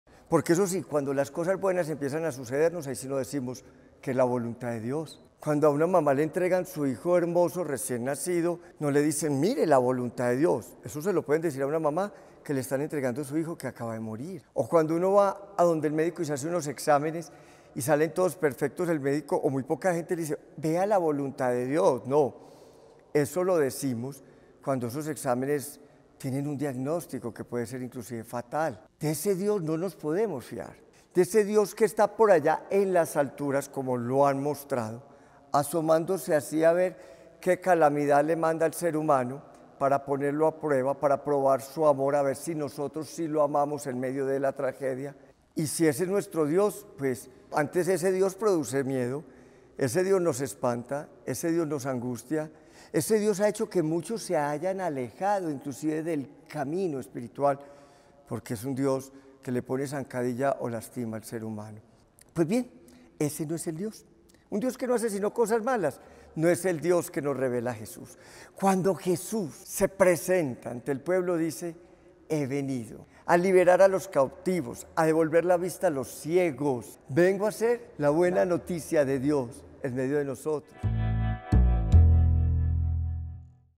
¿Cuál es tu Dios? / ¿Un Dios bueno o malo? - El Buen Dios
¿Alguna vez te has preguntado dónde está Dios en medio de los desafíos y las alegrías de la vida? En este intrigante video, nos sumergiremos en una profunda reflexión sobre la presencia divina en nuestro mundo y en nuestras experiencias cotidianas.
Juntos, exploraremos la pregunta universal: ¿Dónde está Dios? ¡Únete a nosotros en esta trascendental búsqueda de significado y conexión espiritual!